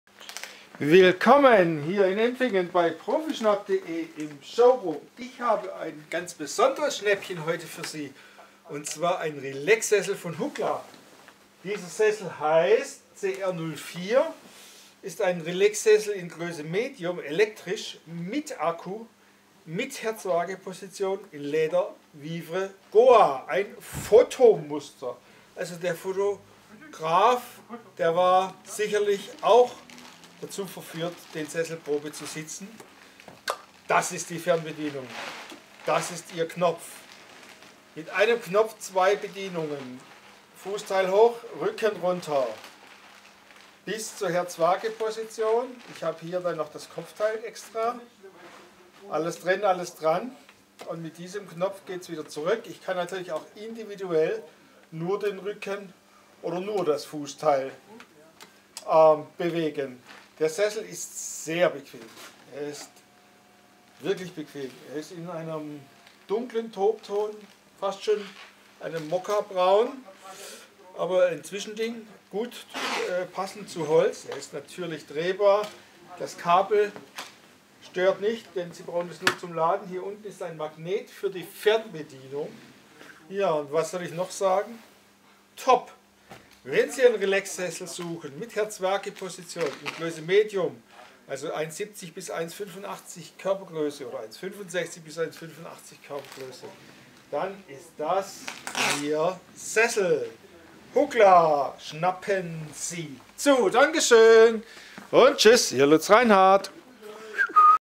HUKLA CR06 Relaxsessel elektr Herz Waage small Leder smoke
Der Relaxsessel mit Herz-Waage-Position wurde...

... auf der Messe so erstmals vorgestellt: Luxus pur. Er kam zuerst ins 1b-Warenlager von Hukla nähe Diepenau, von dort zu uns. Die leichte Formgebung mit der grazilen offenen Armlehne ist langlebig und ein optisches Highlight, sie lässt sich mit vielen anderen Möbelstücken ideal kombinieren. Der Markenbeschlag mit getrennter elektrischer Verstellmöglichkeit von Rücken, oberer Rücken  und Fußteil macht es Ihnen leicht, Ihre Lieblingsposition zu finden. Alles funktioniert auf Knopfdruck mit einer sehr übersichtlichen Fernbedienung.   Sie kommen bis in die Herz-Waage-Position, der Sitzschwerpunkt senkt sich ab: für eine optimale Blutzirkulation.  Der Semianilinleder-Bezug in Spitzen-Qualität ist pflegeleicht und hautsymphatisch.  Mit Lederpflegeset von Keralux für Semianilinleder (auch bei uns erhältlich) einfach zu reinigen. Männer- und kindergeeignet!

Wenn Sie ein hochwertigen Relaxsessel mit vollelektrischer Verstellmöglichkeit bis zur Herz-Waage-Position in einer gut zu kombinierenden  Farbe und in einem pflegeleichten Leder suchen, sind Sie hier genau richtig. Der Relaxsessel wird Ihr Lieblingsplatz im Wohnzimmer. Der Sessel ist Größe "small", ideal für Personen bis 170 cm Körpergröße.